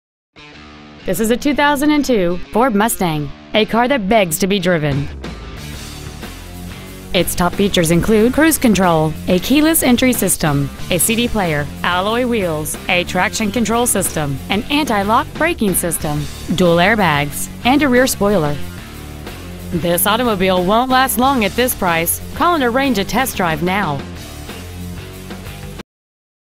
2002 Ford Mustang Matteson IL
Year : 2002
 Make : Ford
 Model : Mustang
 Engine : 3.8
 Trans . : Automatic
 Exterior : Yellow
 Miles : 89,127
 Interior : Black
 Stock : 1614A

 25963 S. Governors Hwy
  , IL 60449

 Pre-Owned 2002 Ford Mustang  IL